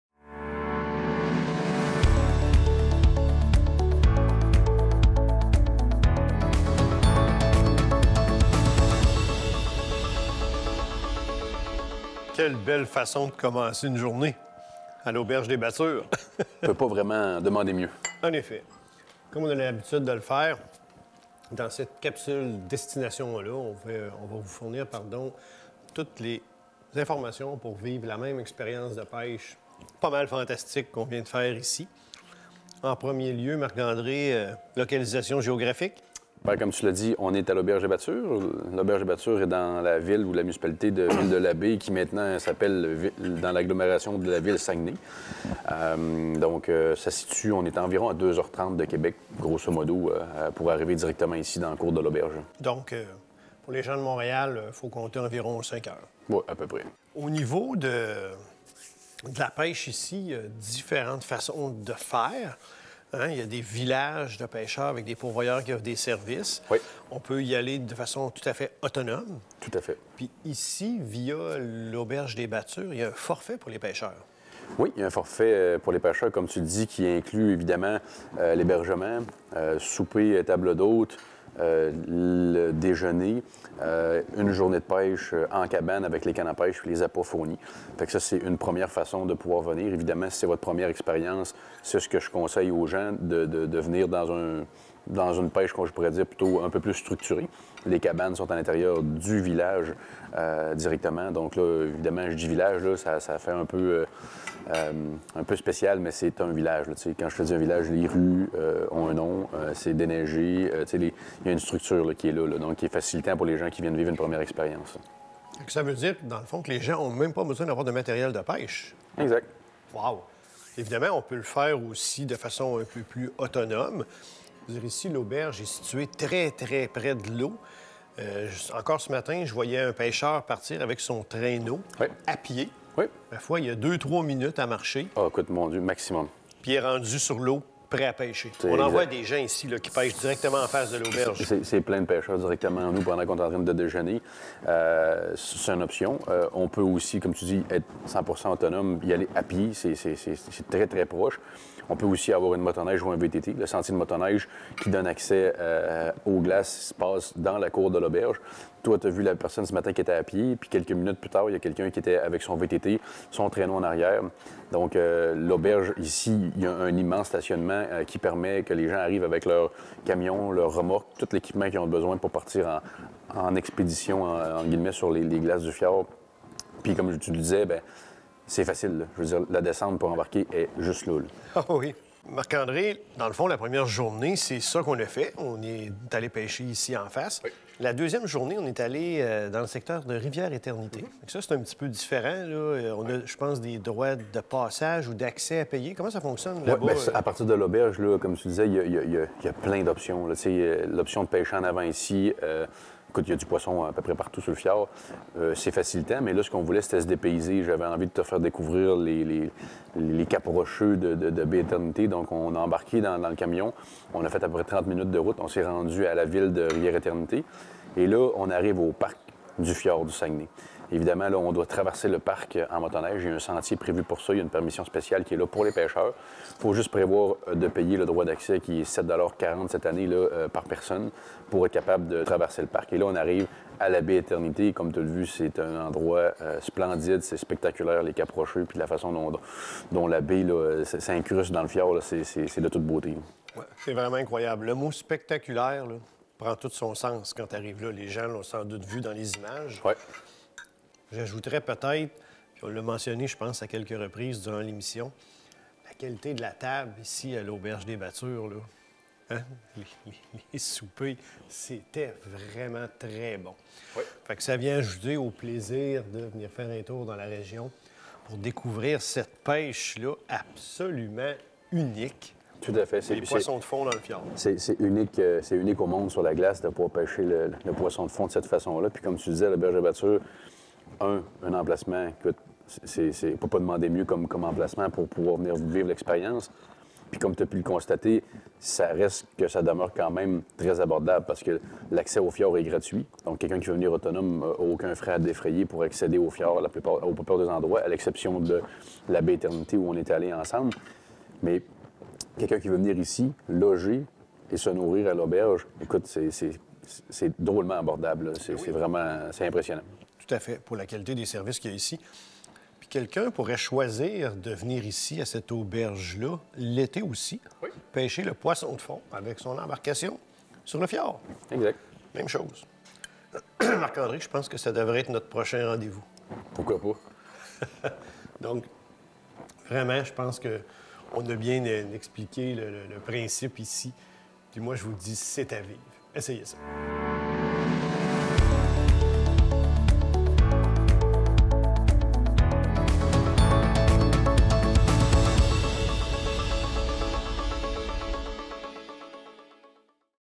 Capsule Destination - Fjord du Saguenay
Marc-André et Daniel vous parle de cette destination de pêche tout à fait majestueuse!